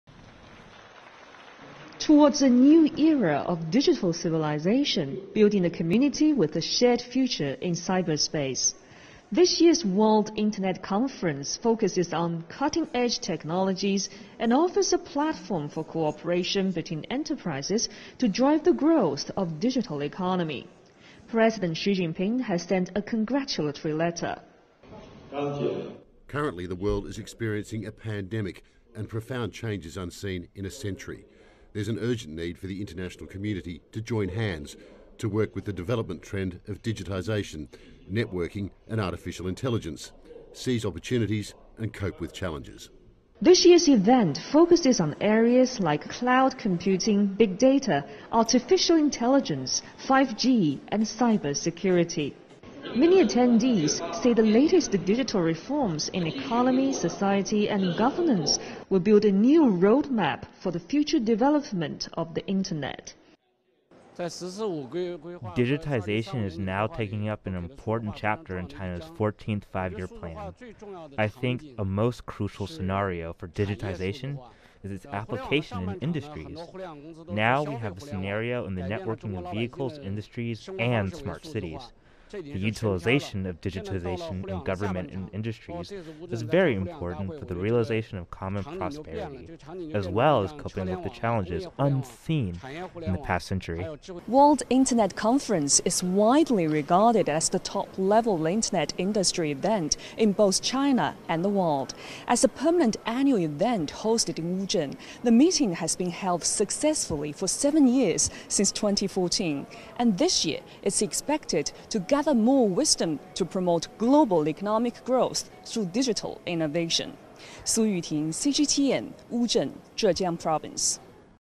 World Internet Conference: Digital era calls for global cooperation to explore fresh impetus
The 2021 World Internet Conference has opened in the ancient town of Wuzhen, in East China's Zhejiang Province. Some 2,000 representatives from domestic and foreign governments, international organizations, industry associations, global leading enterprises, universities and research institutes from over 80 countries and regions, are scheduled to attend the Conference.